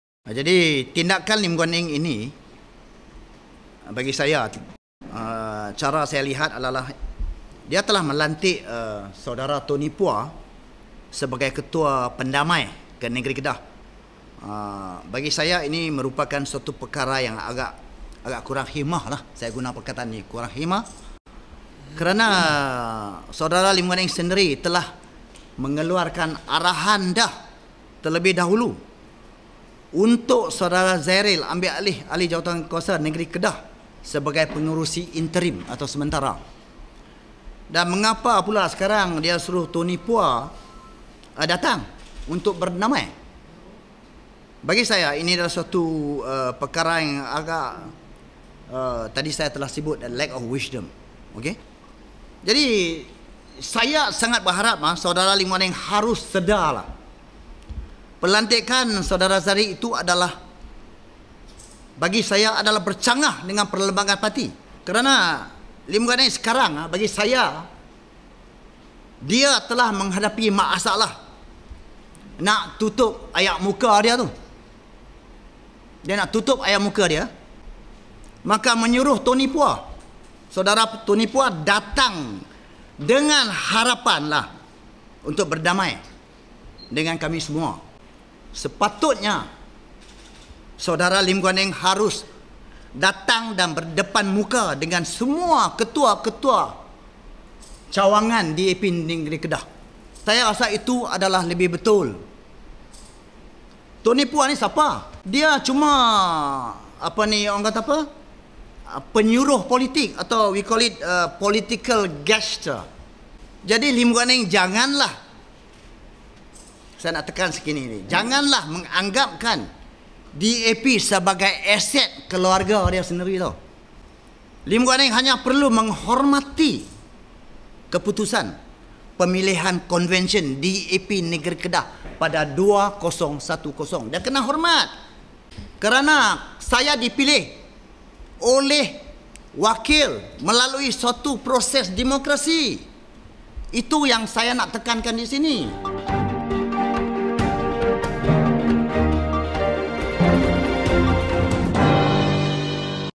Guan Eng Perlu Turun Sendiri Selesai Krisis DAP Kedah
Setiausaha Agung DAP Lim Guan Eng diminta untuk turun sendiri menyelesaikan krisis antara parti diperingkat negeri dan pusat dan bukan hanya menghantar beberapa pemimpin pusat termasuk Tony Pua yang bertindak sebagai perantara.

Pengerusi DAP Kedah Lee Guan Aik berkata, tindakan Guan Eng menggantikan dirinya dengan Zairil Khir Johari untuk mengetuai DAP Kedah adalah tidak munasabah ekoran Guan Aik dipilih sendiri oleh pemimpin di peringkat cawangan.